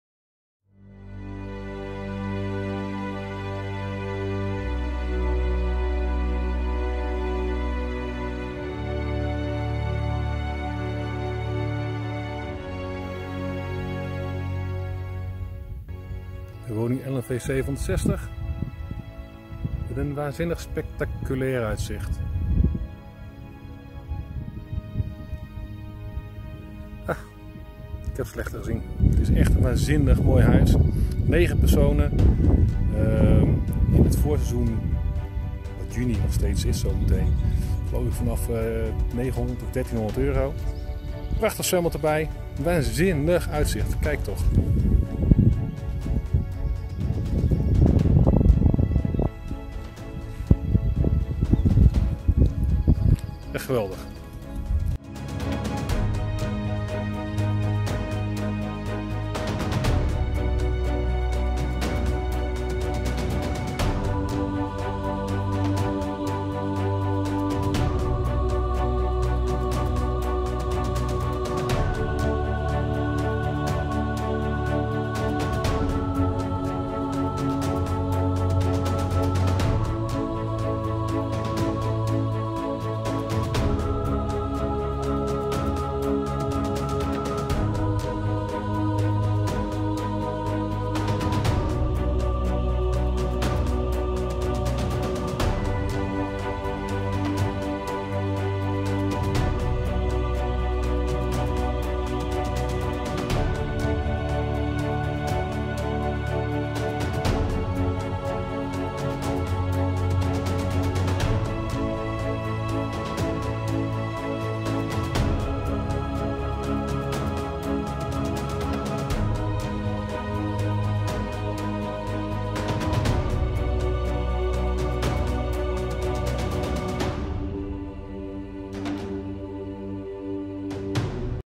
Italie Le Marche vakantievilla voor 8 personen
Vakantievilla in Italië, regio Le Marche. Een echte aanrader. Ik ben hier zelf meerdere keren geweest. En ik het het zeer aanbevelen. Een Top Villa met een geweldig uitzicht! Klik voor meer info